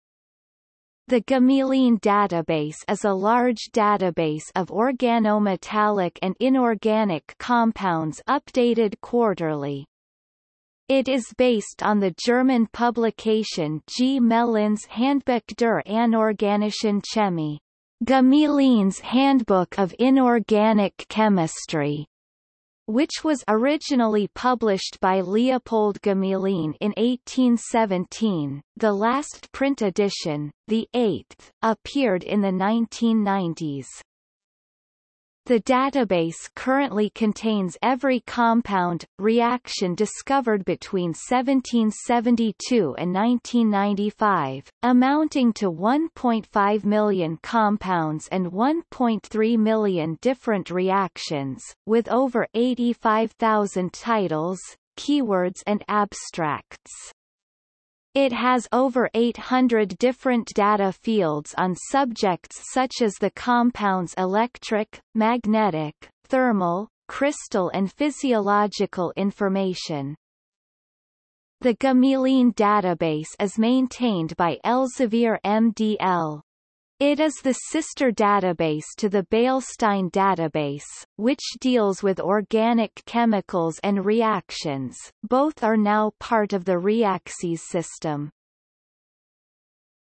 Gmelins Handbuch der anorganischen Chemie | Wikipedia audio article
- Socrates

SUMMARY
The Gmelin database is a large database of organometallic and inorganic compounds updated quarterly. It is based on the German publication Gmelins Handbuch der anorganischen Chemie ("Gmelin's handbook of inorganic chemistry") which was originally published by Leopold Gmelin in 1817; the last print edition, the 8th, appeared in the 1990s.
The database currently contains every compound/reaction discovered between 1772 and 1995, amounting to 1.5 million compounds and 1.3 million different reactions, with over 85,000 titles, keywords and abstracts. It has over 800 different data fields on subjects such as the compounds electric, magnetic, thermal, crystal and physiological information.
The Gmelin database is maintained by Elsevier MDL. It is the sister database to the Beilstein database, which deals with organic chemicals and reactions; both are now part of the Reaxys system.